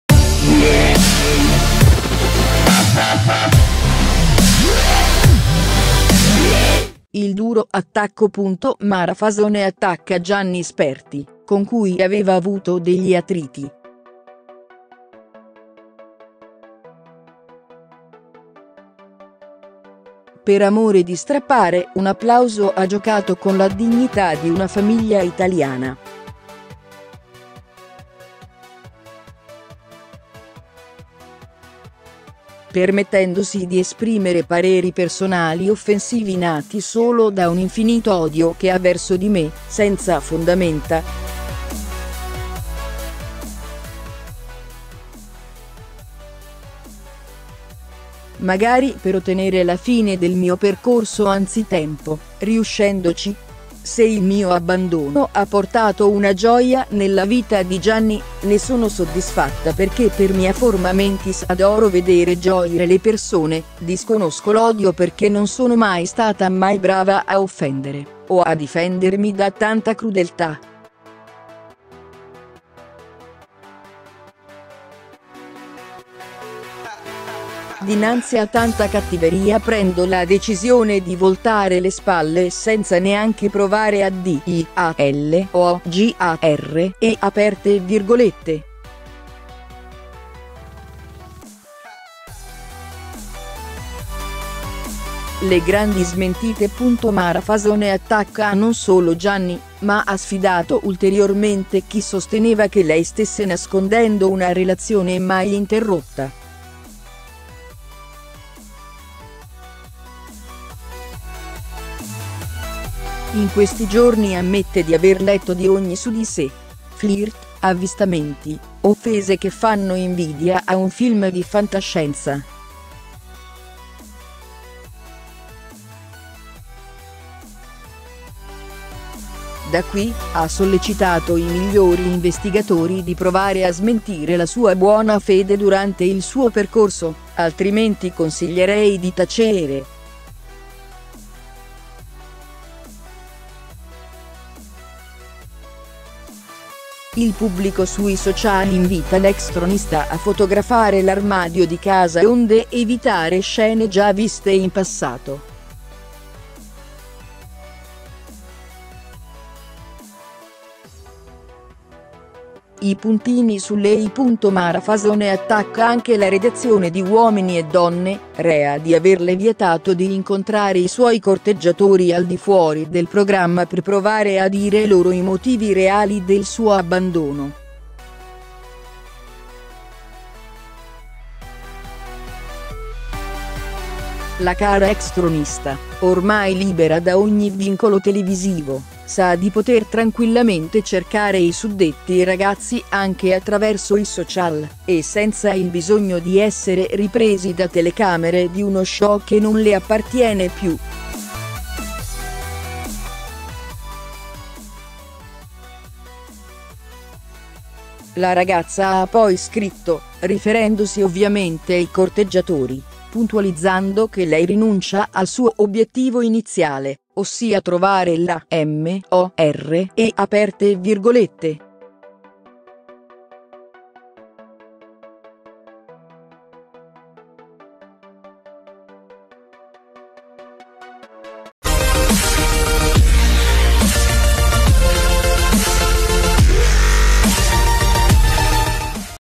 Uomini e Donne: Mara Fasone attacca duramente Gianni Sperti | La prove del notizia
Il duro attacco
Mara Fasone attacca Gianni Sperti, con cui aveva avuto degli attriti:

“Per amore di strappare un applauso ha giocato con la dignità di una famiglia italiana. Permettendosi di esprimere pareri personali offensivi nati solo da un infinito odio che ha verso di me, senza fondamenta. Magari per ottenere la fine del mio percorso anzitempo… riuscendoci! Se il mio abbandono ha portato una gioia nella vita di Gianni, ne sono soddisfatta perché per mia forma mentis adoro vedere gioire le persone, disconosco l’odio perché non sono mai stata mai brava a offendere o a difendermi da tanta crudeltà. Dinanzi a tanta cattiveria prendo la decisione di voltare le spalle senza neanche provare a dialogare“.

Le grandi smentite
Mara Fasone attacca non solo Gianni ma ha sfidato ulteriormente chi sosteneva che lei stesse nascondendo una relazione mai interrotta. In questi giorni ammette di aver letto di ogni su di sè: flirt, avvistamenti, offese che fanno invidia a un film di fantascienza. Da qui, ha sollecitato i migliori investigatori di provare a smentire la sua buona fede durante il suo percorso, altrimenti consiglierei di tacere. Il pubblico sui social invita l’ex tronista a fotografare l’armadio di casa onde evitare scene già viste in passato.

I puntini sulle ‘i’
Mara Fasone attacca anche la redazione di Uomini e Donne, rea di averle vietato di incontrare i suoi corteggiatori al di fuori del programma per provare a dire loro i motivi reali del suo abbandono.

La cara ex tronista, ormai libera da ogni vincolo televisivo, sa di poter tranquillamente cercare i suddetti ragazzi anche attraverso i social e senza il bisogno di essere ripresi da telecamere di uno show che non le appartiene più. La ragazza ha poi scritto, riferendosi ovviamente ai corteggiatori, puntualizzando che lei rinuncia al suo obiettivo iniziale, ossia “trovare l’amore“. Gossip, # LaVita24h,